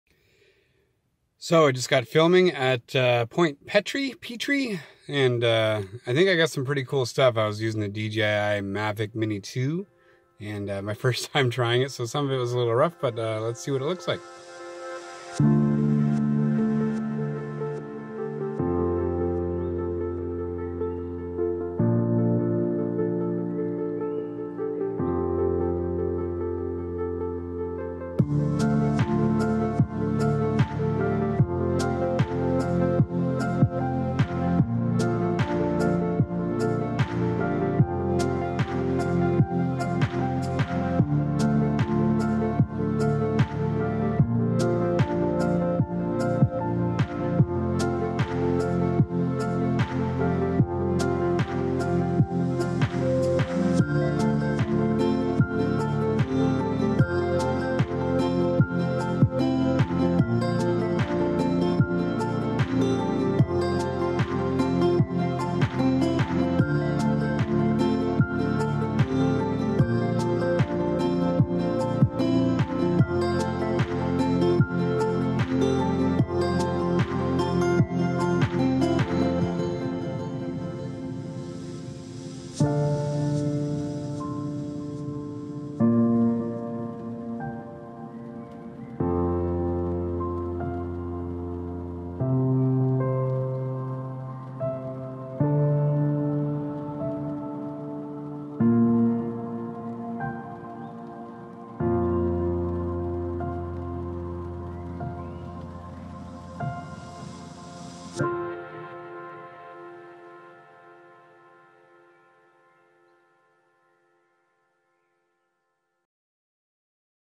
FIRST TIME EVER TRYING A DRONE (DJI Mavic Mini 2)
I got myself a new toy! I had a lot of fun filming throughout the afternoon on such a beautiful day. Many more of these to come.